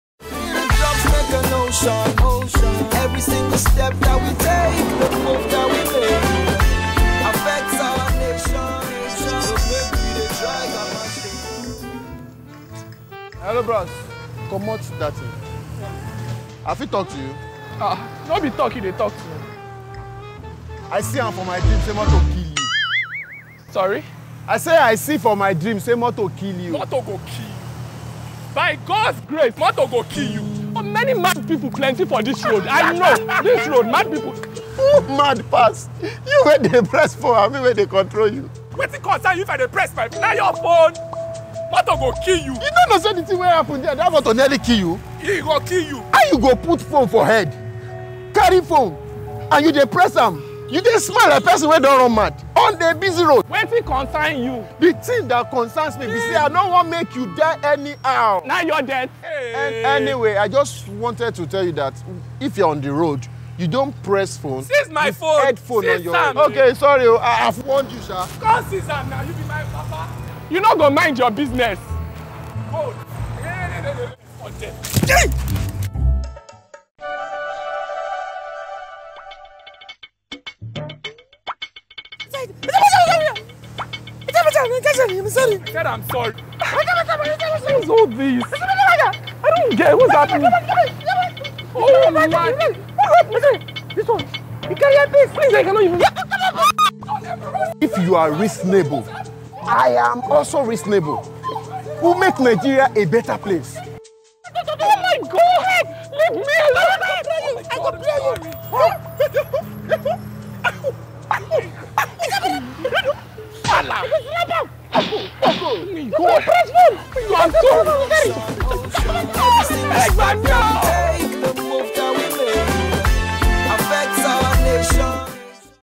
Phone Distraction (Grip Nigeria)
Can you safely text and listen to music while walking at the same time? The distraction from listening and texting while walking can pose great dangers to pedestrians on the road.

It is incredibly dangerous specifically for millennials, be the change and let it start with you. Be reasonable and Nigeria will be a better place for you and I.